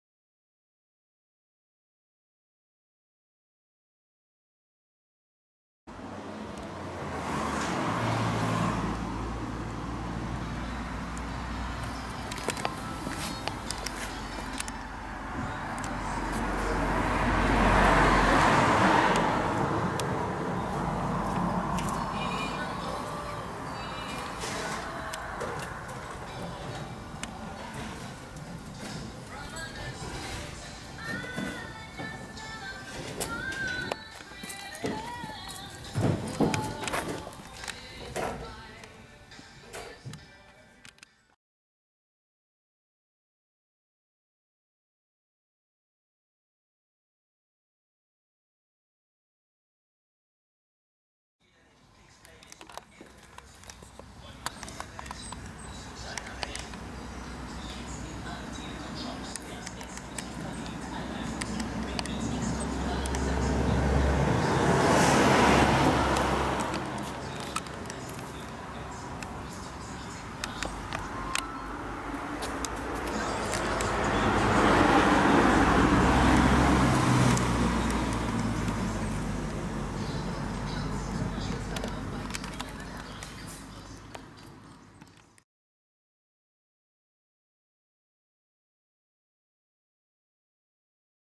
BMW M5 (E39) - Before / After Engine Cleaning - Stuttgart
A BMW M5 (E39) before and after a professional engine cleaning parked in a district of Stuttgart (Germany):

- 400 HP
- 500 Nm max. torque
- 250 (296) km/h topspeed
- 5.3 sec. (0 to 100 km/h)